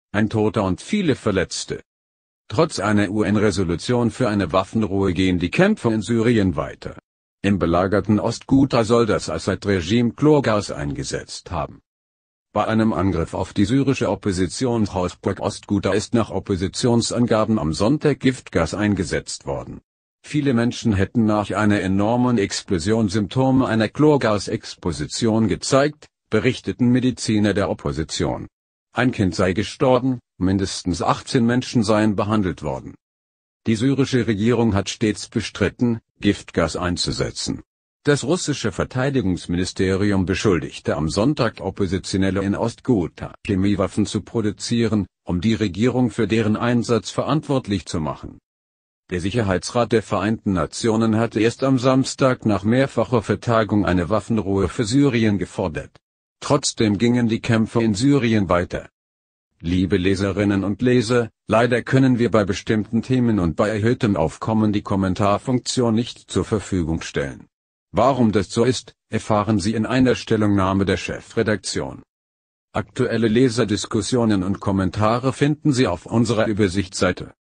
Syrien: Opposition berichtet von Giftgas-Angriff in Ost-Ghuta
Ein Toter und viele Verletzte
Trotz einer UN-Resolution für eine Waffenruhe gehen die Kämpfe in Syrien weiter. Im belagerten Ost-Ghuta soll das Assad-Regime Chlorgas eingesetzt haben.
Bei einem Angriff auf die syrische Oppositionshochburg Ost-Ghuta ist nach Oppositionsangaben am Sonntag Giftgas eingesetzt worden.